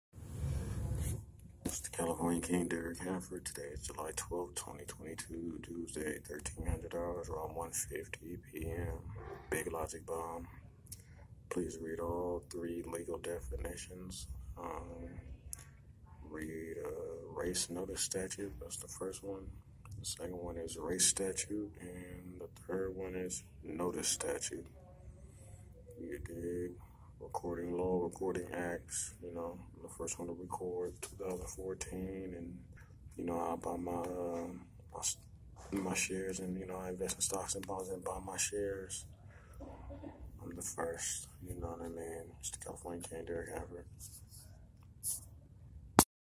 This is a Big Logic-Bomb please read the legal definitions in the description box!!! Big-Logic-Bomb
race-notice statute. (1968) A recording law providing that the person who records first, without notice of prior unrecorded claims, has priority. About half the states have race-notice statutes. Also termed race-notice act; notice-race statute.

race statute. (1944) A recording act providing that the person who records first, regardless of notice, has priority. Only Louisiana and North Carolina have race statutes. -- Also termed pure race statute; race act.

notice statute. (1864) A recording act providing that the person with the most recent valid claim, and who purchased without notice of an earlier, unrecorded claim, has priority. About half the states have notice statutes. -- Also termed notice act.

Legal definitions from Black's Law Dictionary 9th Edition.